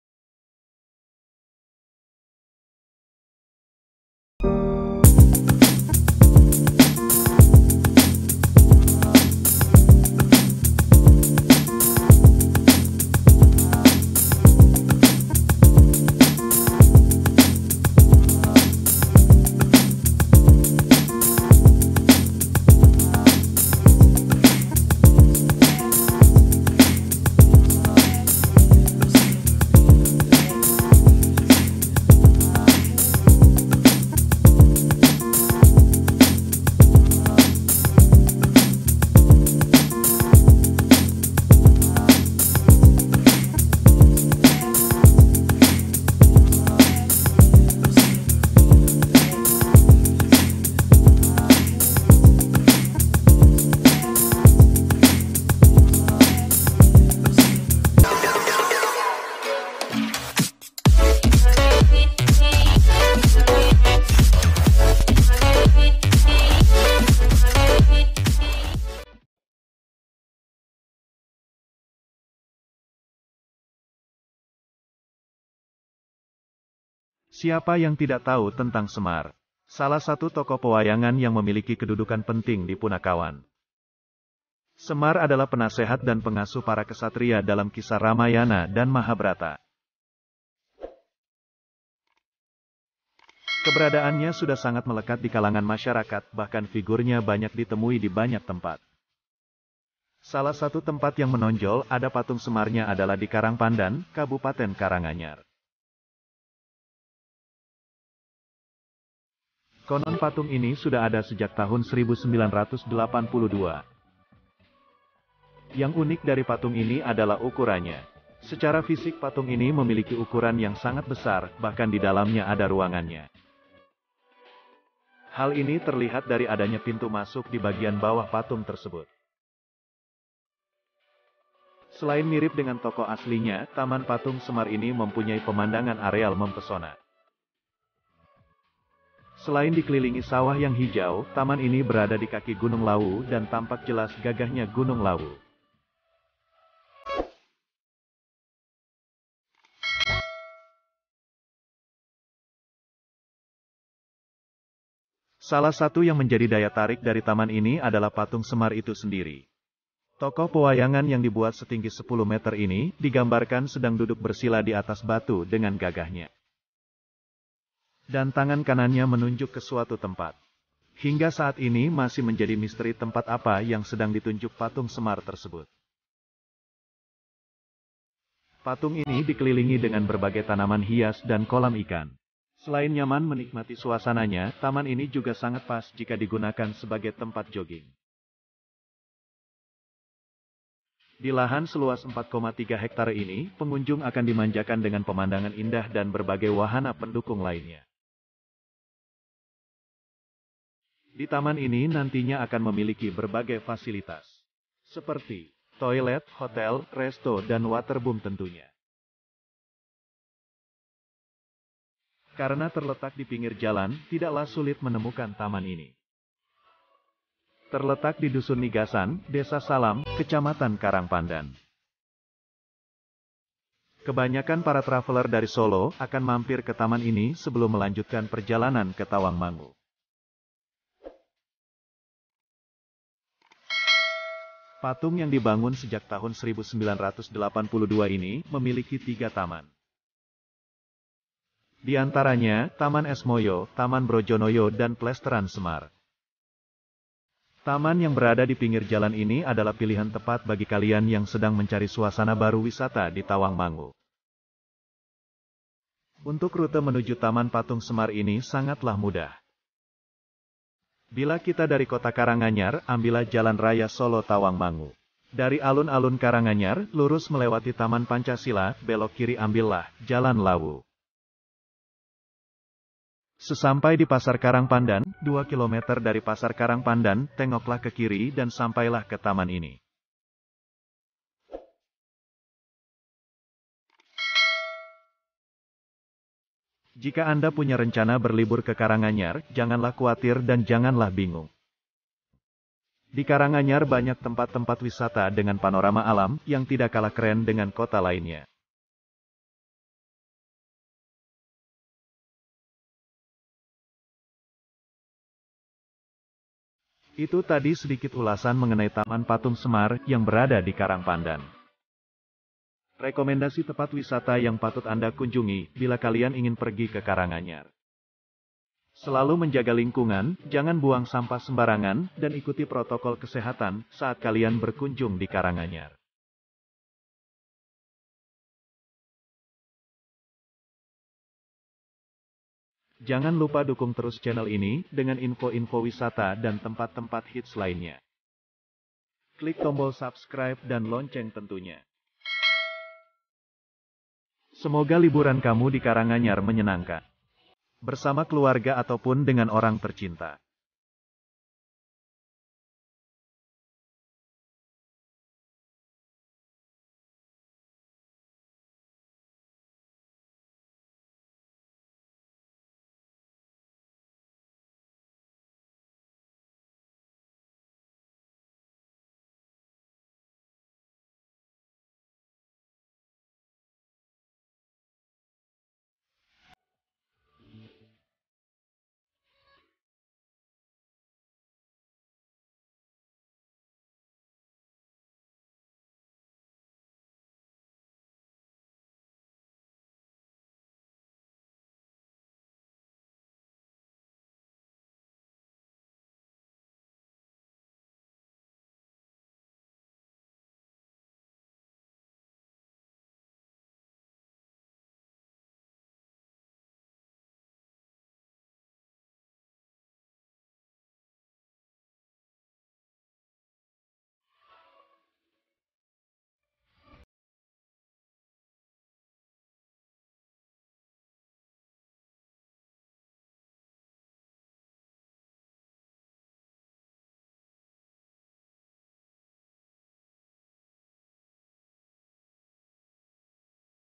Wajah Baru TAMAN PATUNG SEMAR Karangpandan Karanganyar || Video Drone 2021
Wajah baru taman patung semar karangpandan kabupaten karanganyar. Taman yang berada di pingir jalan raya solo tawangmangu ini berlatar belakangkan gagahnya gunung lawu. Selain berlatar belakangkan gunung lawu taman ini memiliki 3 taman.
Penasaran seperti apa tamannya?

📍Lokasi:
▪︎Gedangan, Gerdu, Kec. Karangpandan, Kabupaten Karanganyar, Jawa Tengah